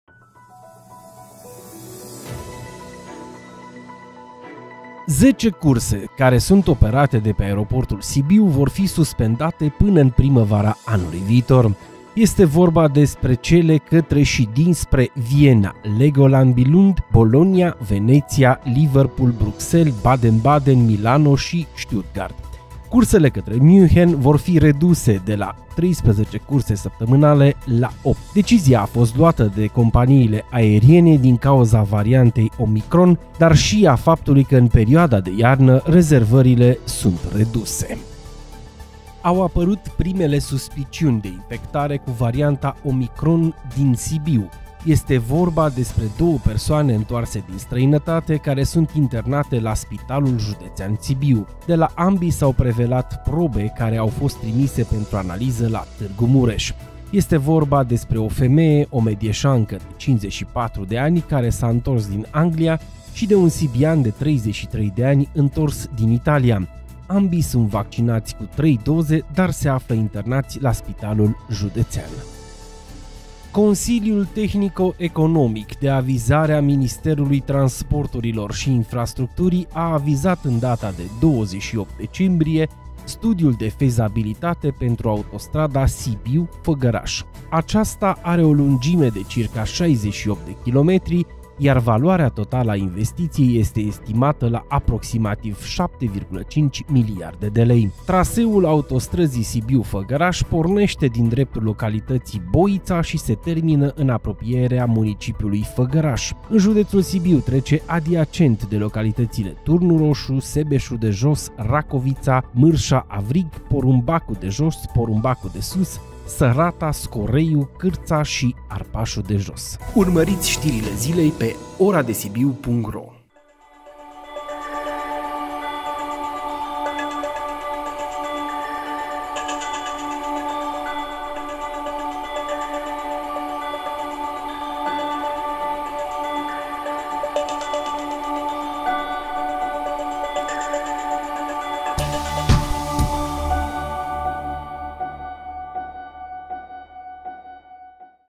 Știrile zilei la Sibiu 🗞 Zece zboruri suspendate temporar de la Sibiu ✈️
Zece zboruri suspendate temporar de la Sibiu ✈️

Primele suspiciuni de Omicron din Sibiu 😷

A fost avizat studiul de fezabilitate al autostrăzii Sibiu - Făgăraş🛣️